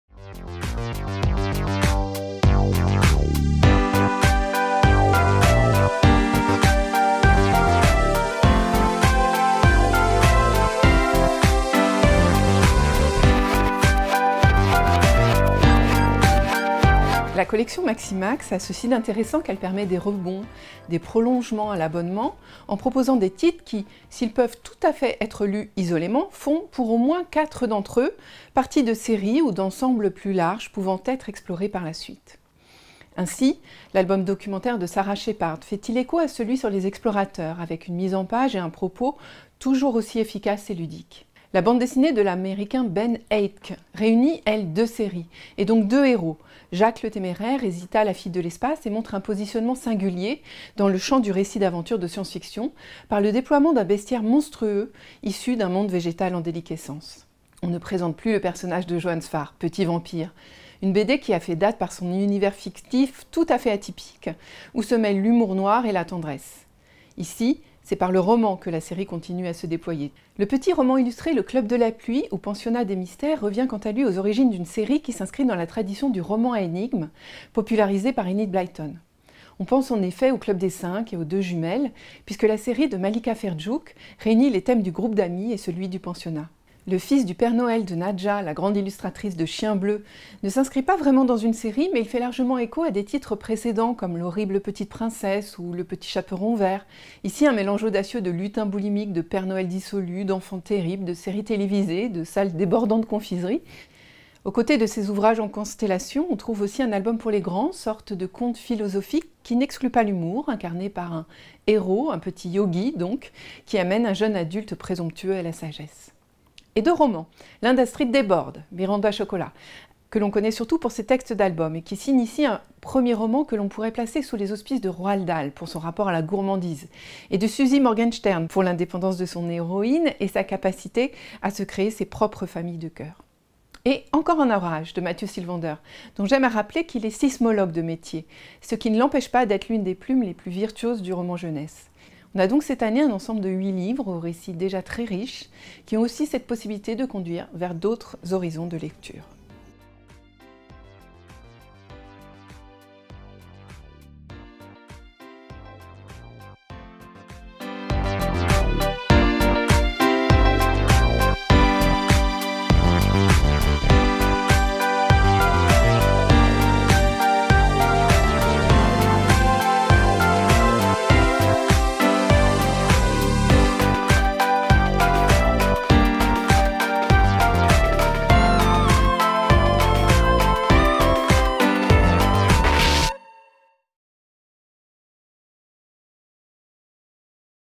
Présentation de la sélection de livres Maximax 2021/2022
Découvrez la sélection des 8 livres Maximax 2021/2022